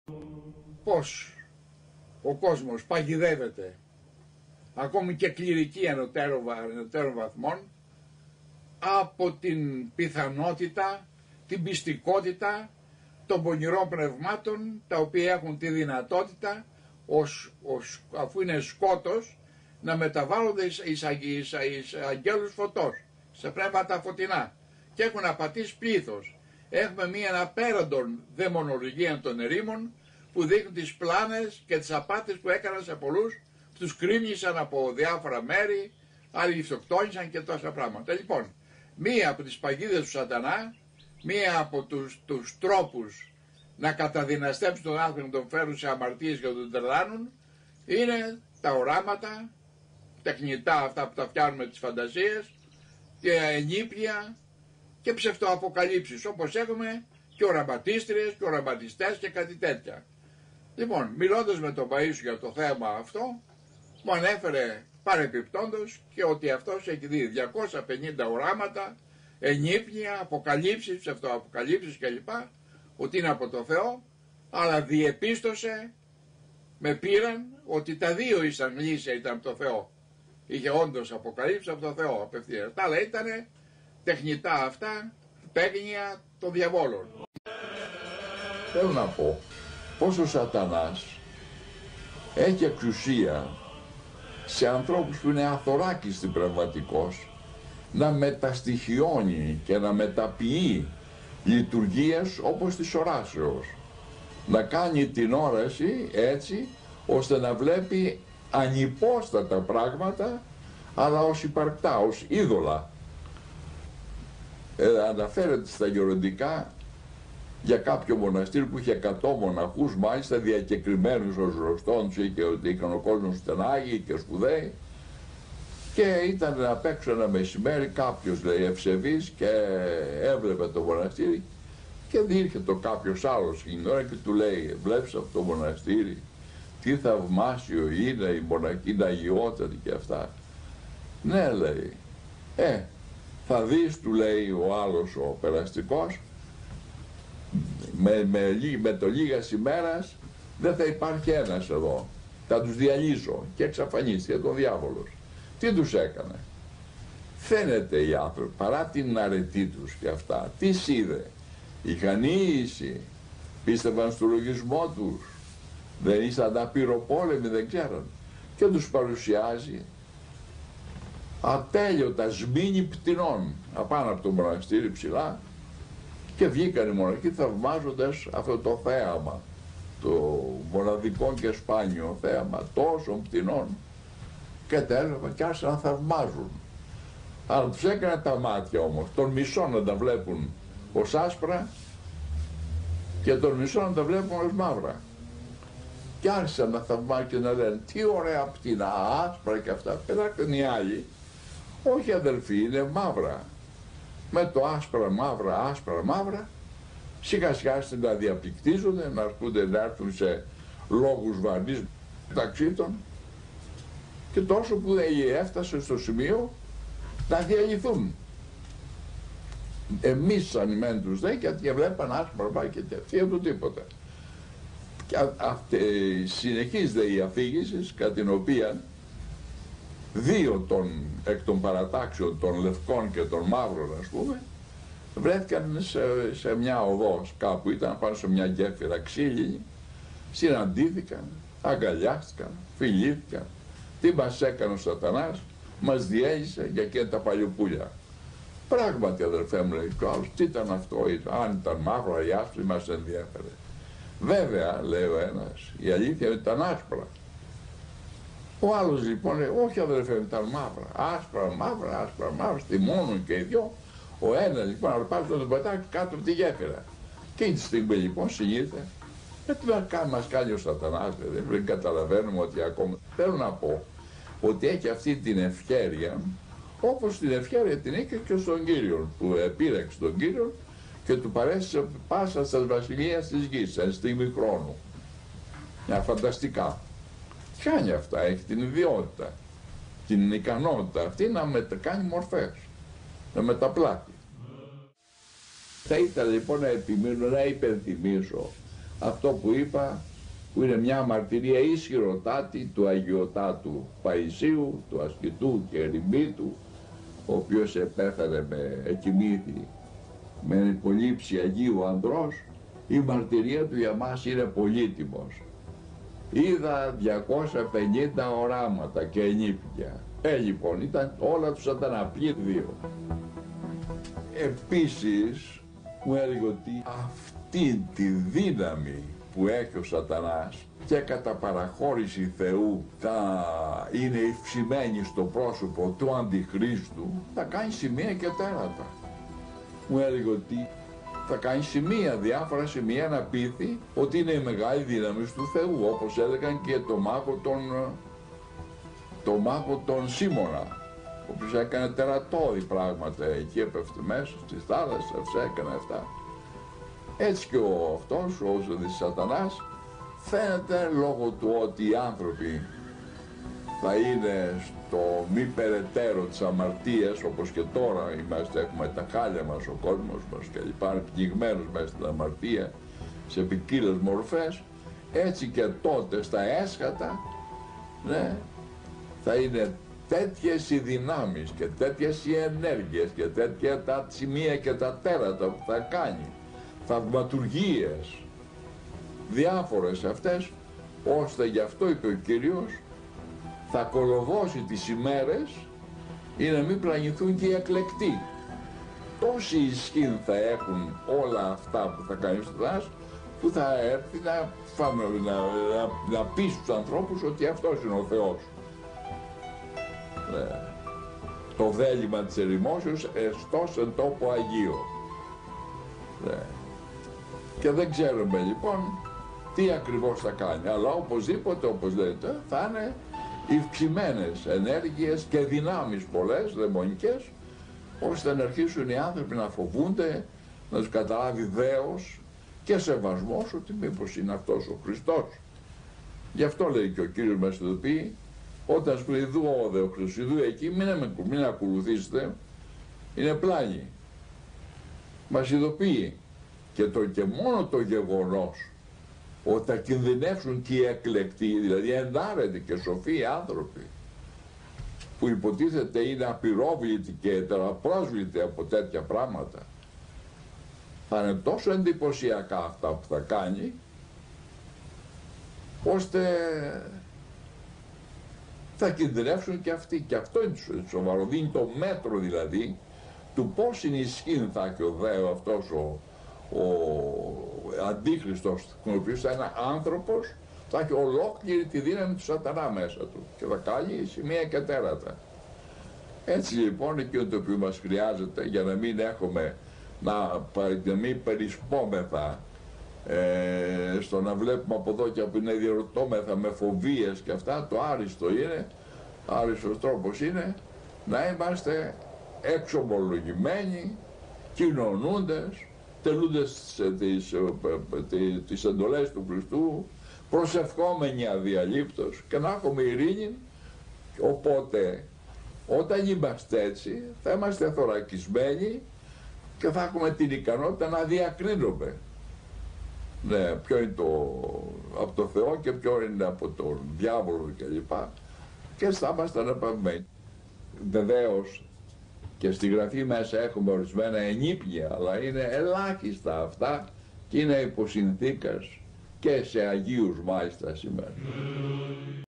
ΣΑΤΑΝΙΚΕΣ ΠΑΓΙΔΕΣ. Πως ο διάβολος πλανά τον άνθρωπο ως άγγελος φωτός, με οπτασίες. π. Θεόκλητος.
Σατανικές παγίδες, Ο π. Θεόκλητος ο διονυσιάτης, μας προειδοποιεί για την ικανότητα και τη δύναμη που έχει ο διάβολος κατά παραχώρηση Θεού, ώστε να ρίχνει τον άνθρωπο, σε πλάνες, παρουσιάζοντας οπτασίες, αποκαλύψεις, ενύπνια, θαύματα, προφητείες, οράματα κ.λ.π. Μπορεί ο διάβολος να παρουσιάζεται με τη μορφή φωτεινού αγγέλου, ή αγίου, ή ακόμα και του Χριστού, σε όποιον δεν είναι θωρακισμένος, με το Λόγο του Θεού, τα μυστήρια της εκκλησίας και την ταπείνωση. Αυτά θα κάνει και ο αντίχριστος δια μέσω ακόμα και ανθρώπων της εκκλησίας, οι οποίοι θα πιστέψουν ότι είναι σε θέση να βλέπουν θαύματα, λόγω της υπερηφάνειας τους, Ο ίδιος ο άγιος Παΐσιος είπε στον ομιλητή π. Θεόκλητο ότι, από τα 250 θαύματα που έχει δει, μόνο τα 2 ήταν από το Θεό, όλα τα άλλα ήταν από τον διάβολο για να τον ρίξει στην υψηλοφροσύνη.